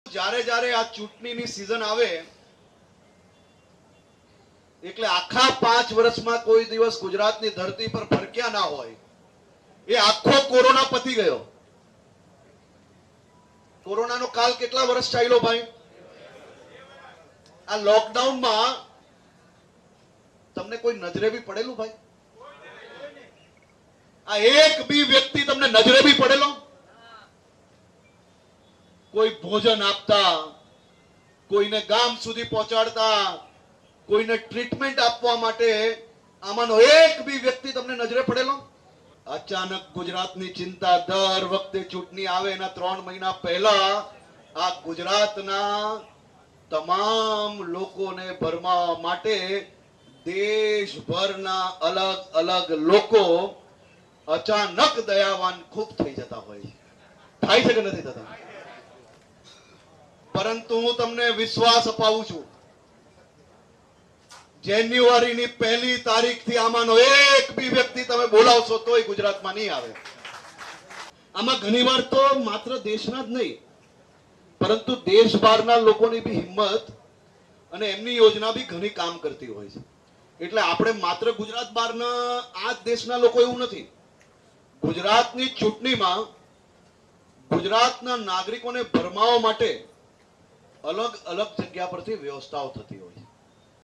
'1 જાન્યુઆરી પછી એક પણ દેખાવાનો નથી', Harsh Sanghavi ના AAP પર પ્રહાર, ડ્રગ્સ અંગે કર્યો હુંકાર
'1 જાન્યુઆરી પછી એક પણ દેખાવાનો નથી', Harsh Sanghavi ના AAP પર પ્રહાર, ડ્રગ્સ અંગે કર્યો હુંકાર | VTV GUJARATI

હર્ષ સંઘવીએ AAP પર પ્રહાર કર્યા હતા. તેમણે ઉમેર્યું કે કેટલાક લોકો ગુજરાતને બદનામ કરવાના કામમાં લાગ્યા છે. પાંચ વર્ષમાં કોઇ દેખાયા નથી. હવે જયારે ચૂંટણી આવે એટલે આવા લોકો આવી જાય છે. અચાનક ગુજરાતની ચિંતા કરવા આવેલા 1 જાન્યુઆરી પછી દેખાવાના નથી અને કોરોના કાળમાં લોકોને જ્યારે જરૂર હતી ત્યારે કોઇ નજરે પડતાં ન હતા તેમ હર્ષ સંઘવીએ ઉમેર્યું હતું.